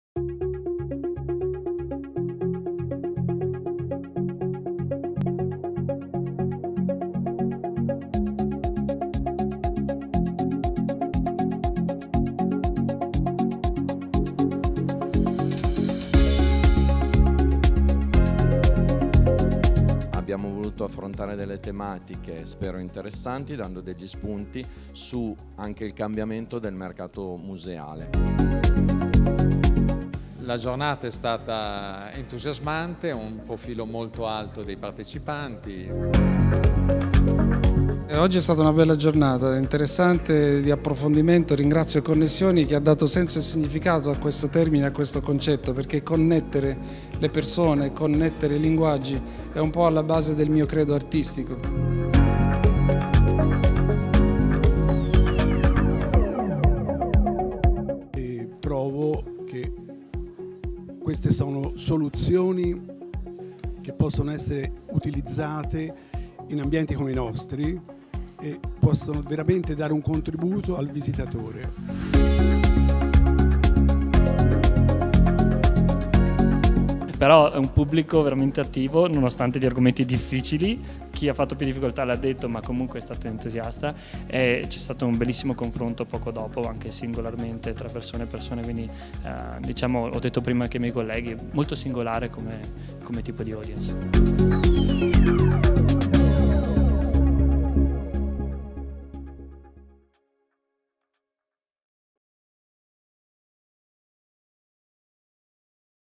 La multimedialità nell'arte - Firenze, 28 giugno 2018
Un video che ripercorre con immagini e interviste la giornata di studio sull'integrazione AV nel museale organizzata a Firenze da Canon e Ligra, in collaborazione con Connessioni.